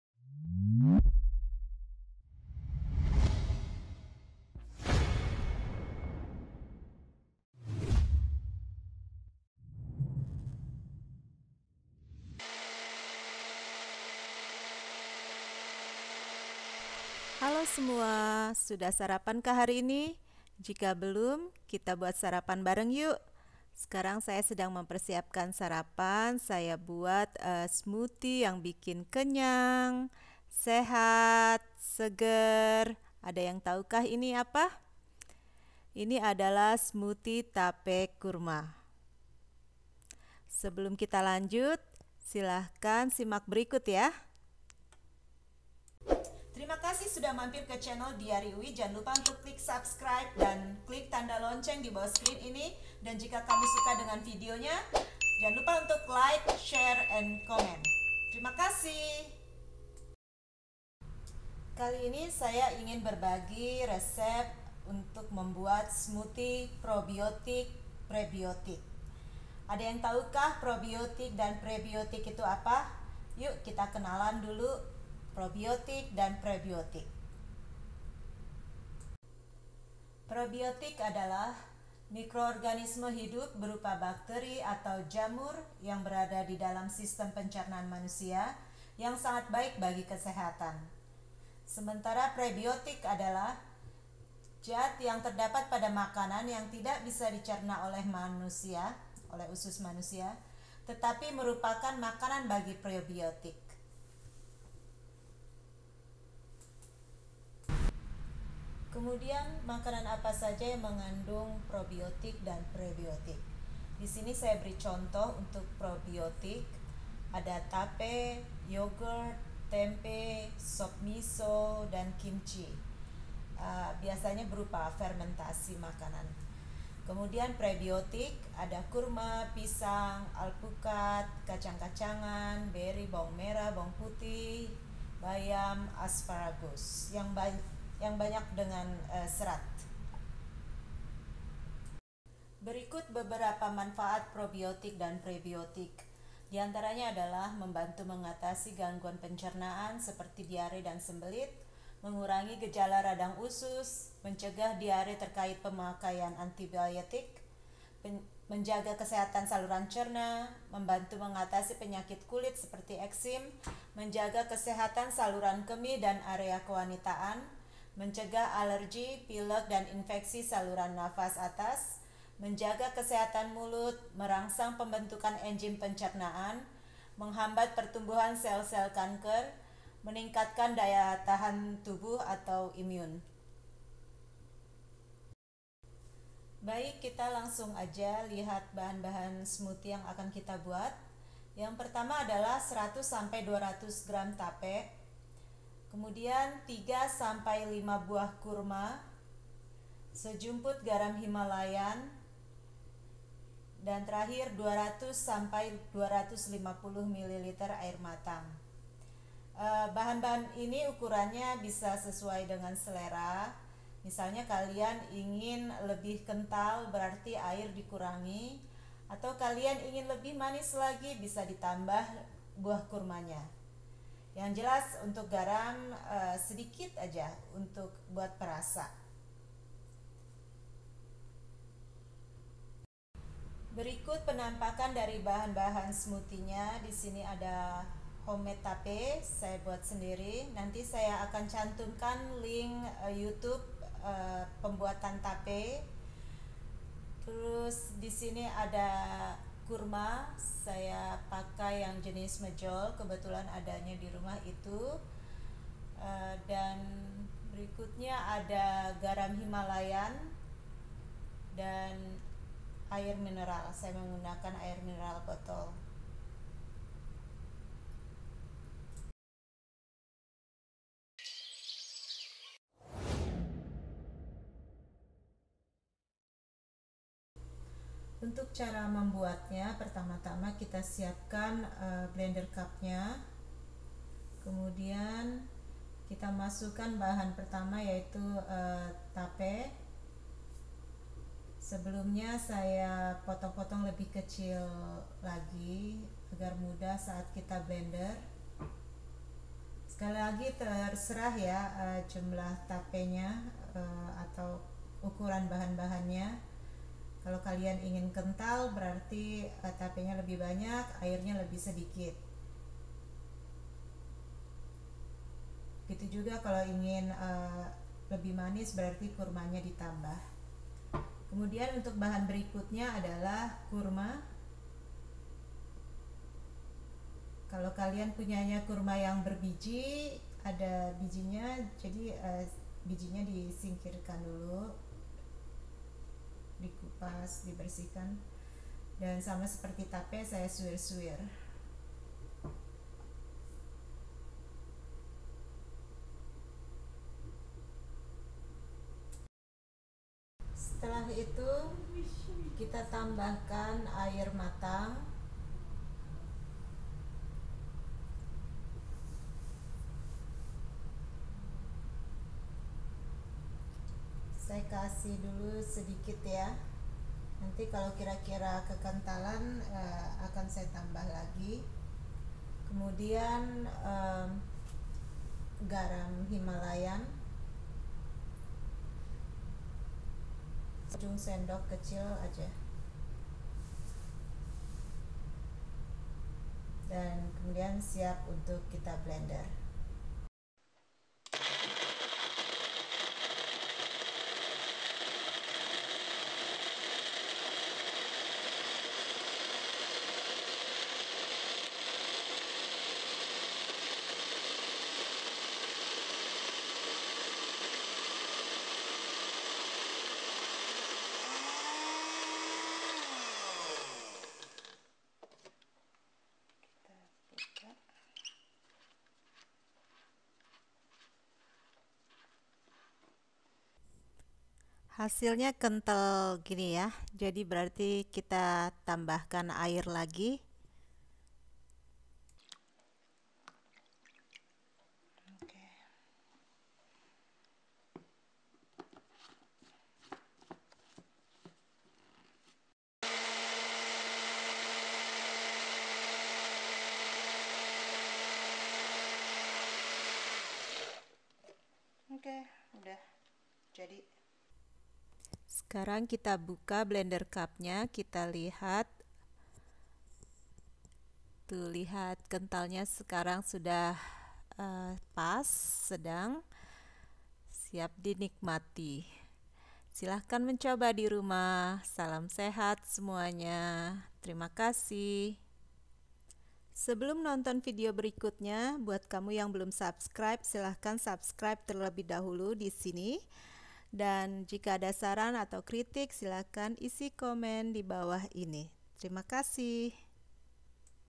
Smoothie Tape Kurma | Date Fermented Cassava Smoothie | Probiotic Prebiotic | Boost Immune
Probiotik - Prebiotik Smoothie
Smoothie Tape Kurma
Bahan-bahan:
100-200 gr Tape

3-5 buah Kurma

Sejumput Garam himalayan

200-250 ml Air matang

Cara Buat:
Blender semua bahan, bisa ditambah air sesuai selera kekentalan smoothie masing-masing.

Probiotic - Prebiotic Smoothies to boost your immune.
Ingredient:
100-200 gr Fermented Cassava
3-5 Dates
pinch of Himalayan Salt
200-250  ml Water (you can add more water).
Blend all ingredients.